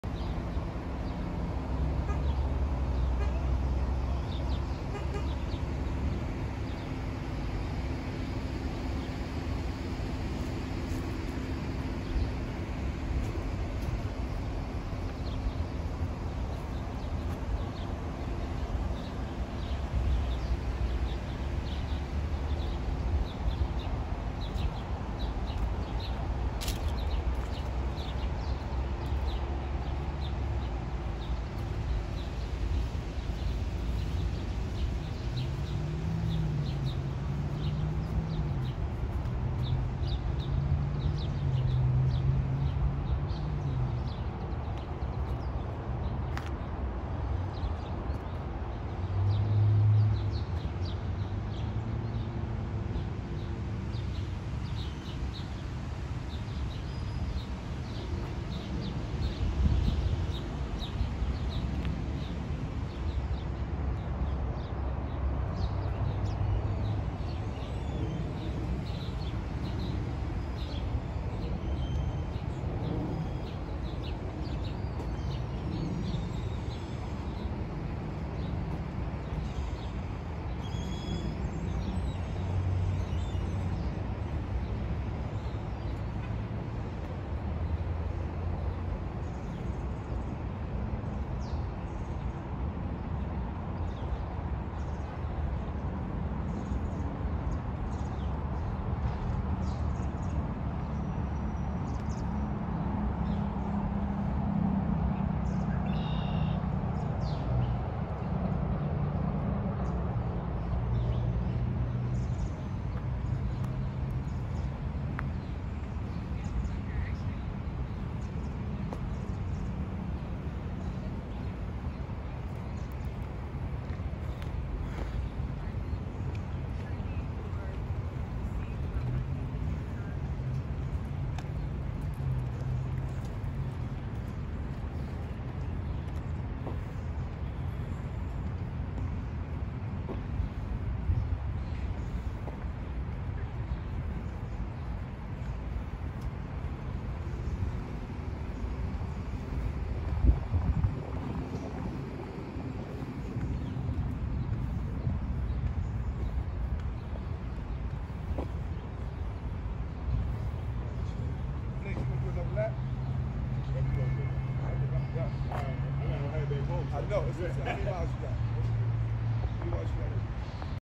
🚣‍♀️ 🏡 Rocky steps, boathouse, Park, Philadelphia
Rocky Steps are amazing in Philadelphia PA, this is a spectacular view from the back of the rocky steps. Also the walking path near the boat house, and there’s so many activities and things to see and do. So many different types of flora and fauna, and it’s quite interesting to see the back of the rocky steps which leads to the boat houses and the park.